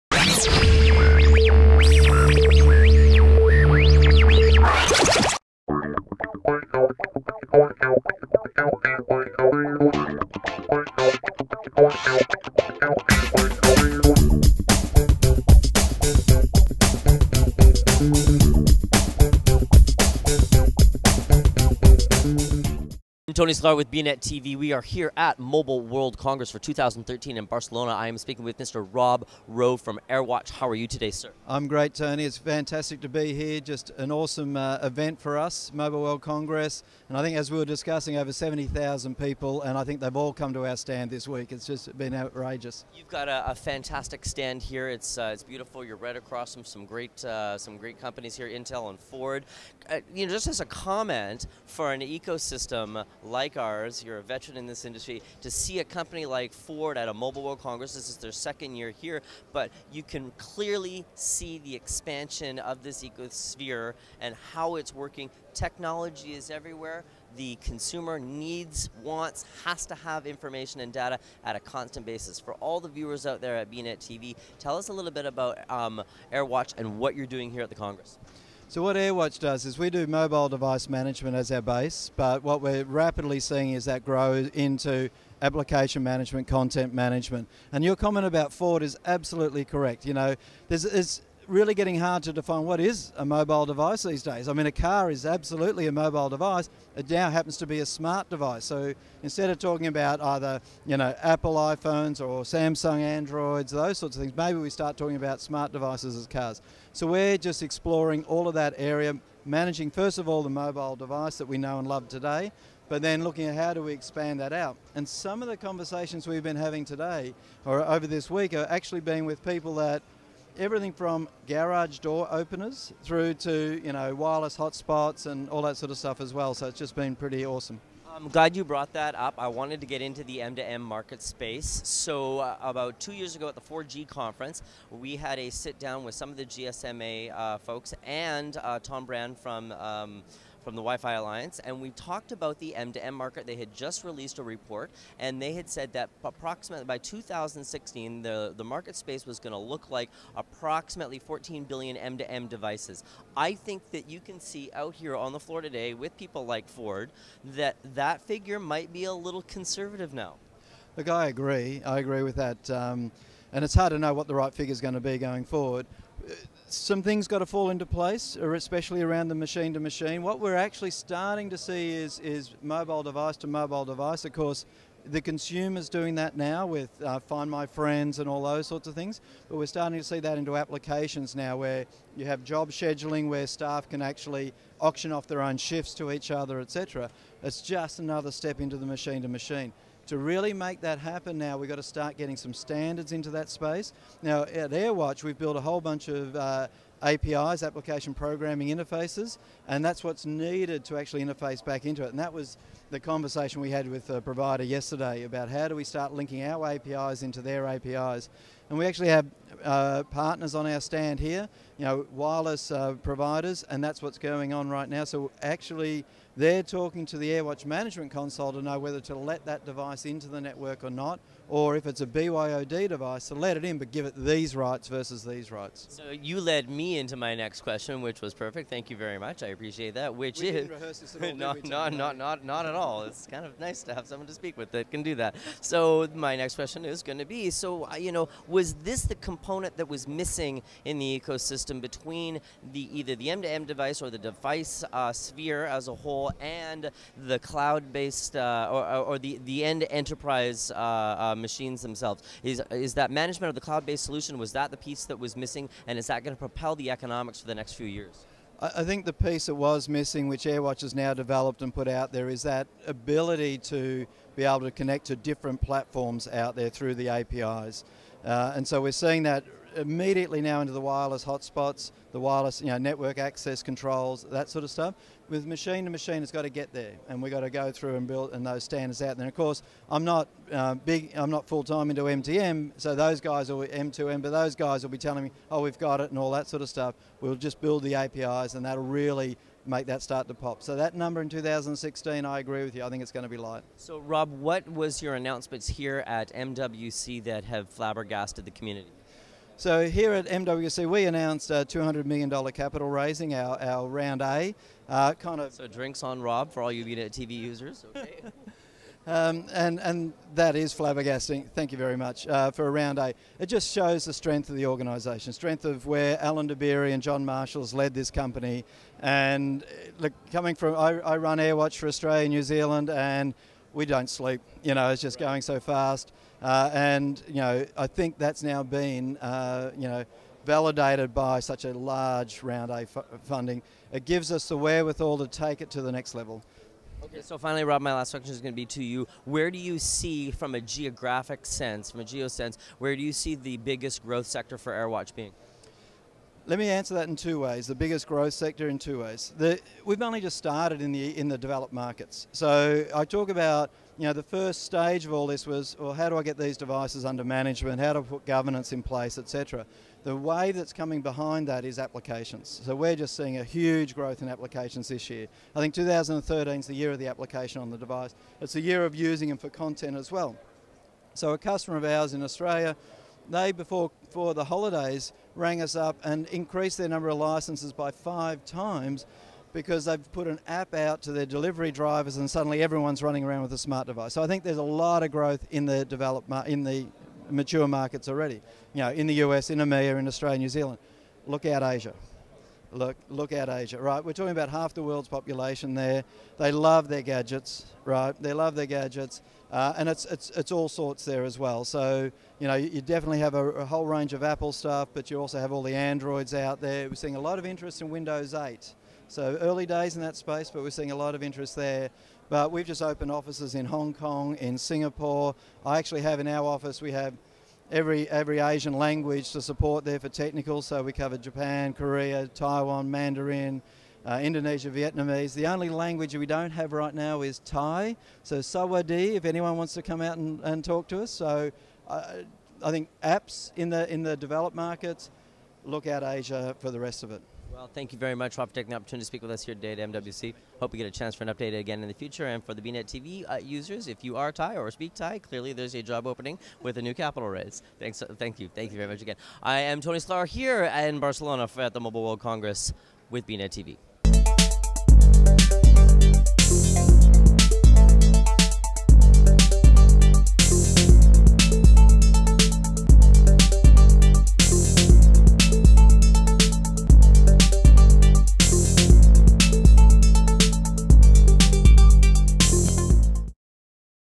AirWatch-Rob Roe
bnetTV speaks with Rob Roe of AirWatch at the 2013 Mobile World Congress show held in Barcelona.